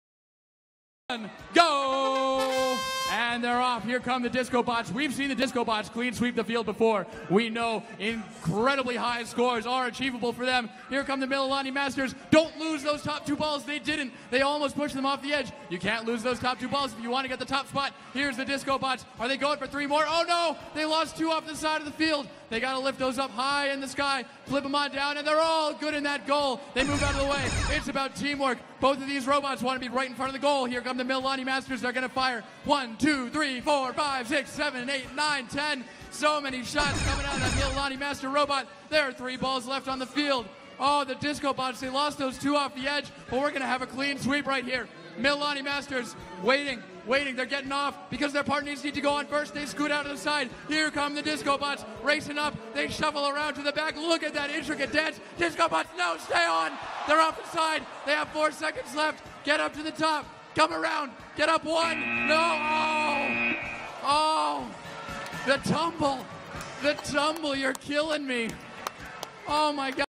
VEX Worlds 2016 - VEXIQ Middle School - Science - Finals 16 (11173 1140A) 152
VEX Worlds 2016 - VEXIQ Middle School - Science - Finals 16

Team Score 152:
11173 - Mililani Masters (Cornerstone Engineering Robotics, Mililani, Hawaii, United States)

1140A - Discobots ?? (Brampton Robotics Education, Brampton, Ontario, Canada)